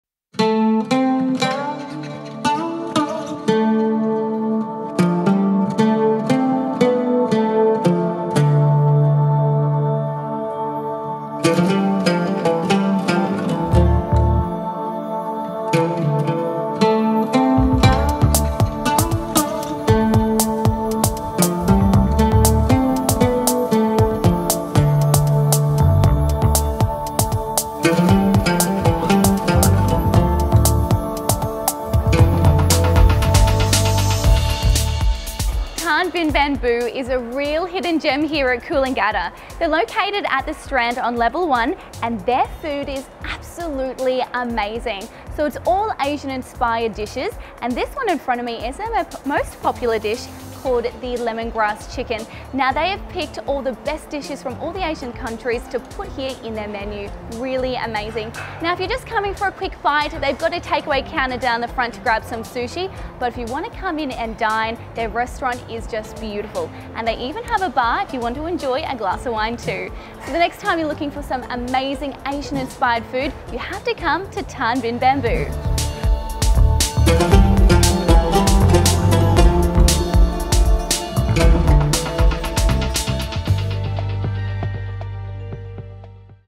Thanh Binh Bamboo Restaurant in Coolangatta serving Japanese food and Chinese cuisine
Inviting you to taste best of Asian fusion in Thanh Binh Bamboo. Take a big bite to excite your taste buds!

Thanh Binh Bamboo
72-80 Marine Parade Coolangatta QLD Australia 4225